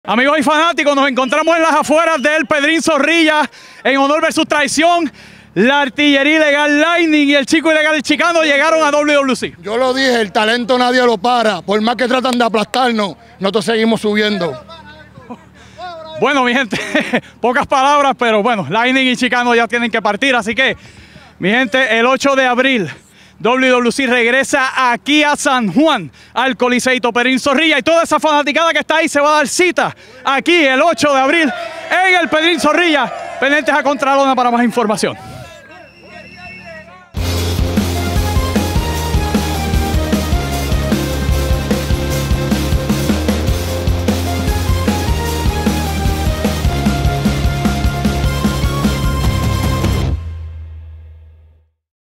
ÚLTIMA HORA: 'La Artillería Ilegal' LLEGA A WWC | Honor vs. Traición
Mira las PRIMERAS DECLARACIONES de 'El Chico Ilegal' El Chicano, junto a Lightning, tras hacer su sorpresivo regreso ANOCHE en WWC Honor vs. Traición en el Coliseito Pedrín Zorrilla de San Juan.